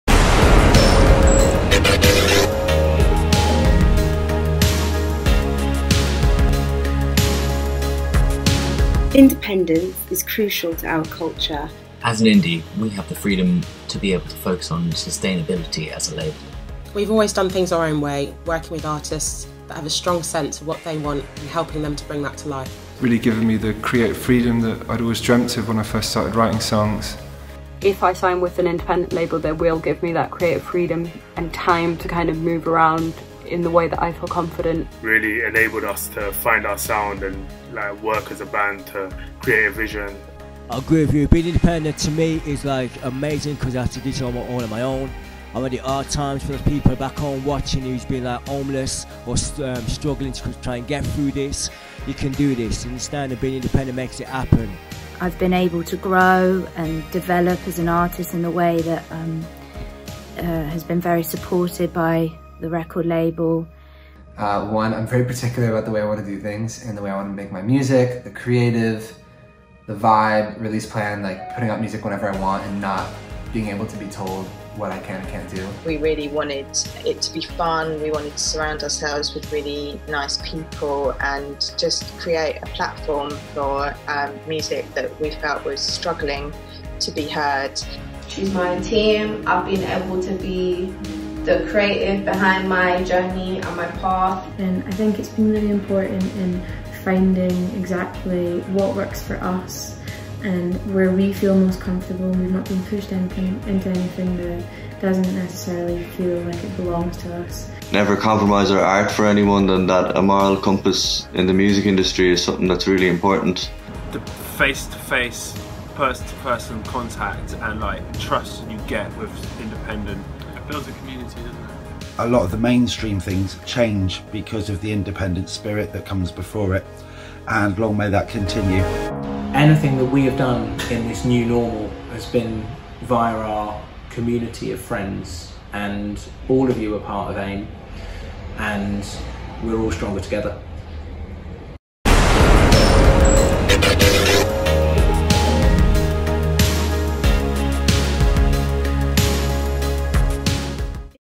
What Does Independence Mean To You - AIM Independent Music Awards 2020
Here are some of the winners and nominees from the AIM Independent Music Awards 2020 discussing why independence is important to them.